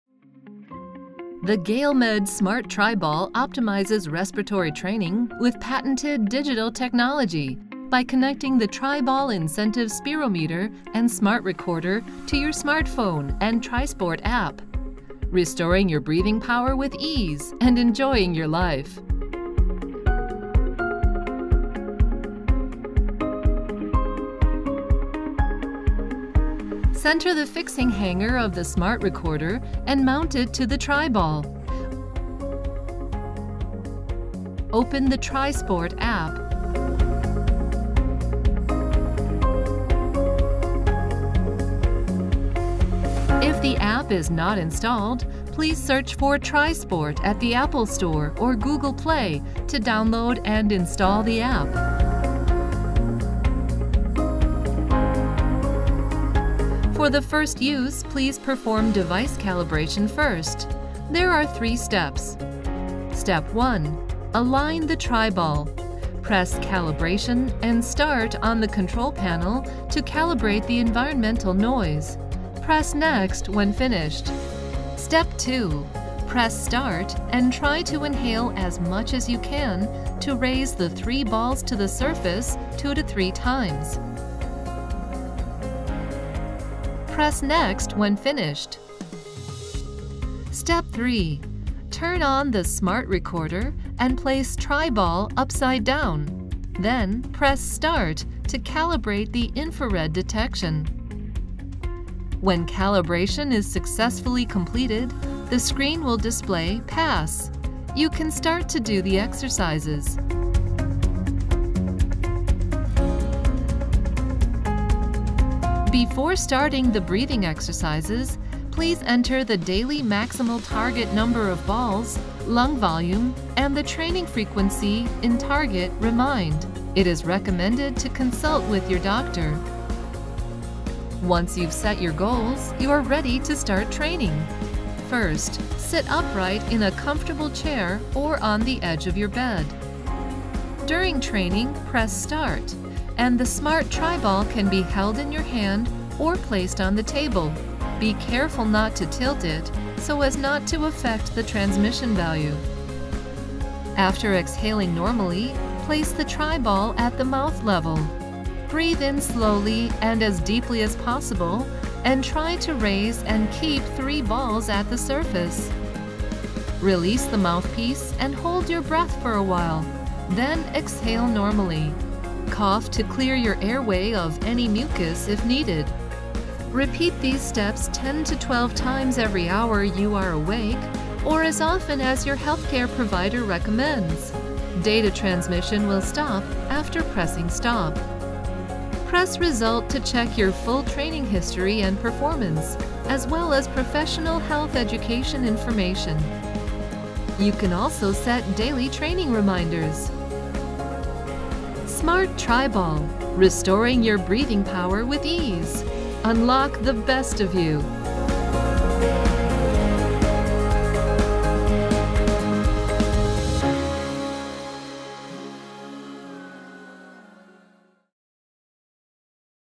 Welcome to the Tri Sport APP! Let’s Start to Use Smart Tri Ball - GaleMed
Let's start to use Smart Tri-Ball!

The GaleMed Smart Tri-Ball optimizes the respiratory training with the patented digital technology by connecting the Tri-Ball Incentive Spirometer and Smart Recorder to your smartphone and “Tri-Sport” APP.
Please search for “Tri-Sport” at the Apple Store or Google Play to download and install the APP.

Follow the video teaching and start your breathing exercise journey.

Restoring your breathing power with ease and enjoying your life.